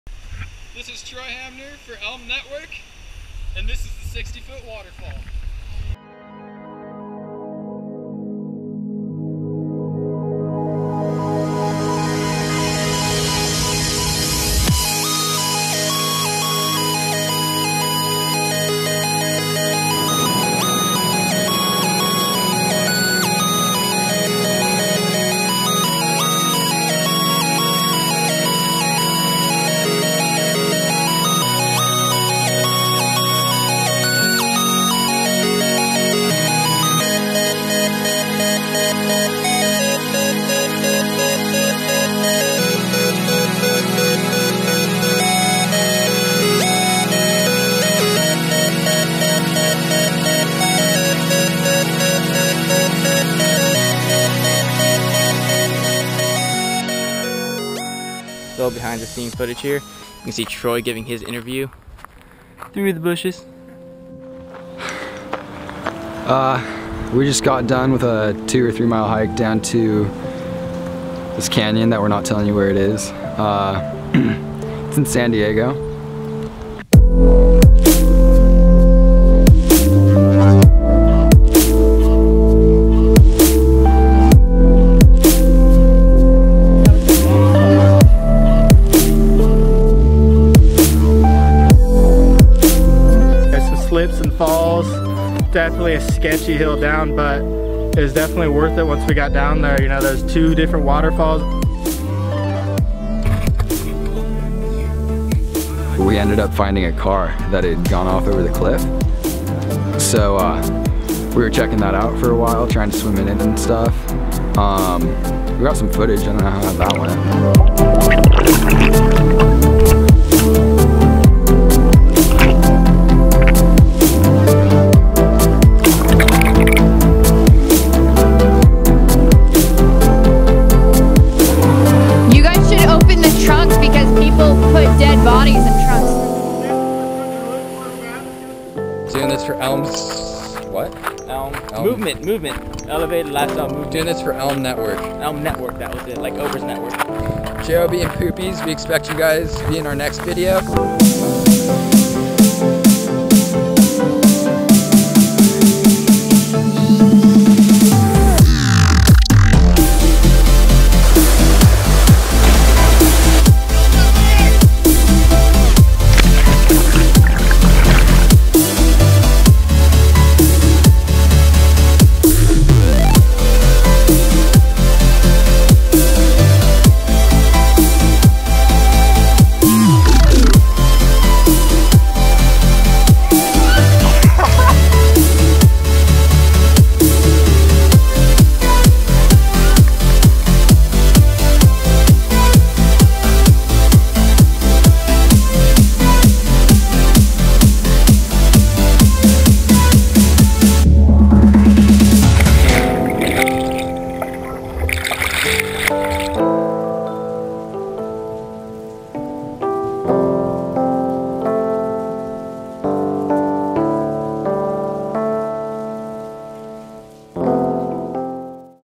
Hidden Falls Cliff Jumping + Surfing | San Diego, CA | Land or Water
The waves were flat so the Land or Water Crew headed to an undisclosed location in San Diego in search of a rumored waterfall perfect for cliff jumping. Watch and check out what they found!

Elevatedlifestylemovement.com